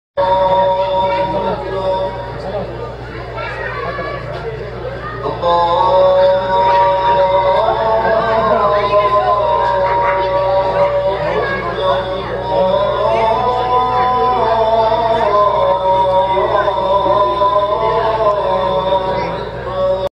HiMY SYeD -- Adhan al Isha, Call to The Night Prayer, Noon Academy, Brampton, May 11 2019
HiMY SYeD -- Adhan al Isha, Call to The Night Prayer, After Community Iftar, Noon Academy, 12 Rutherford Road South, Brampton, Ontario, Canada, Saturday May 11 2019